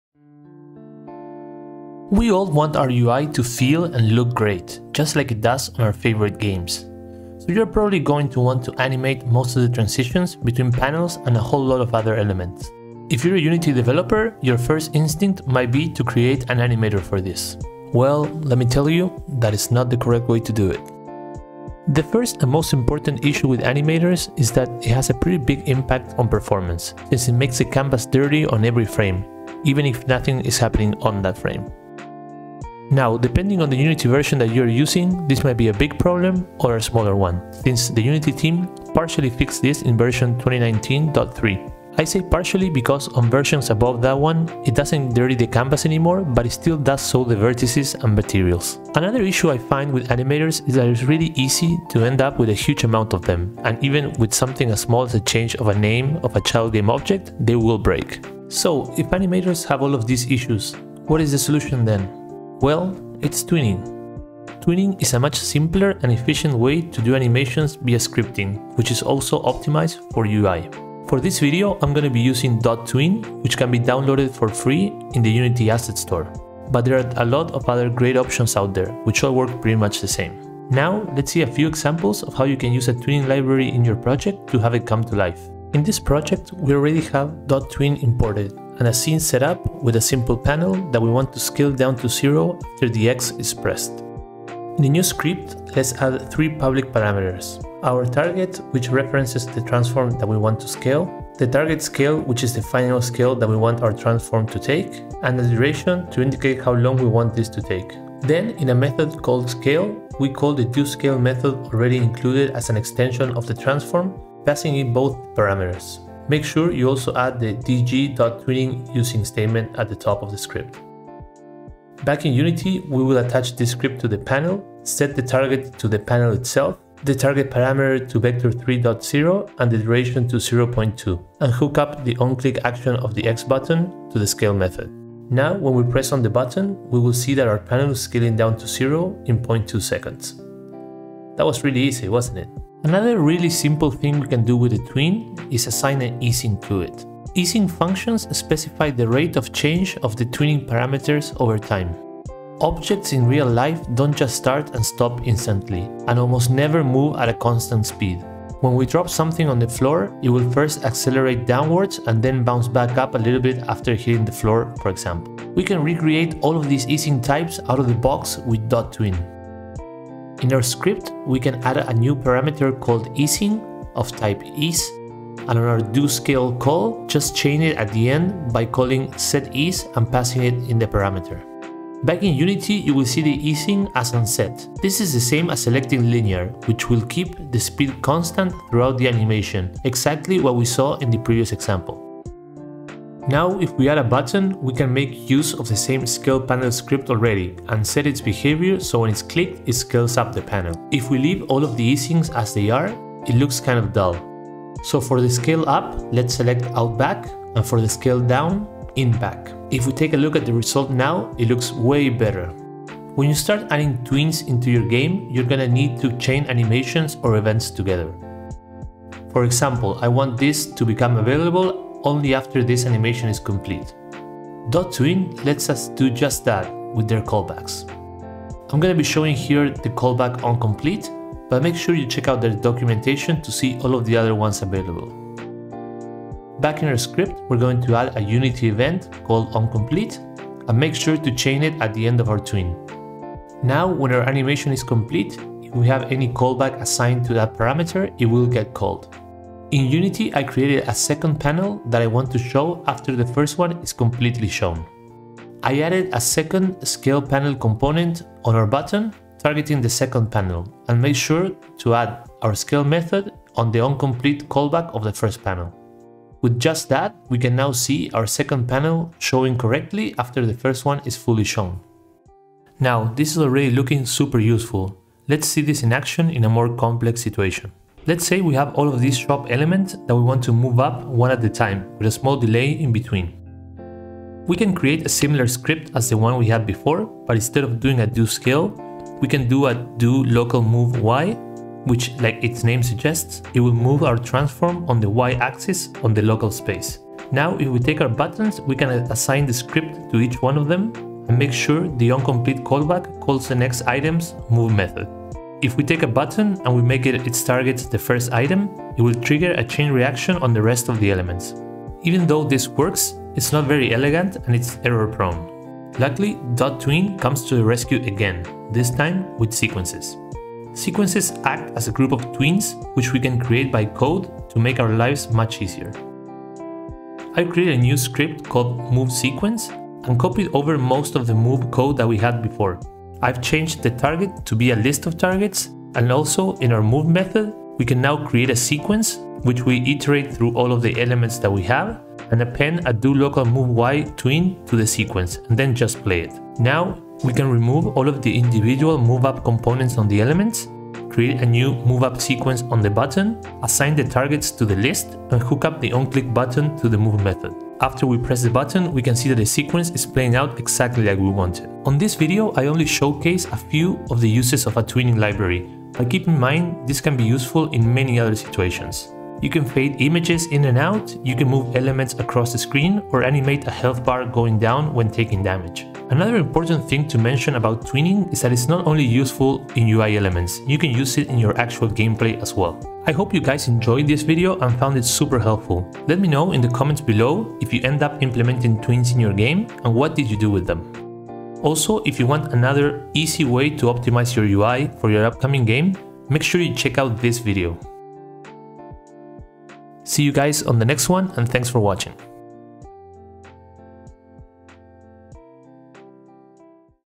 DON'T do this when animating your UI in Unity!
Learn how to correctly animate your UI inside of Unity, increasing the performance of your games.

👍  Also, like if you want to see more videos like this one in the future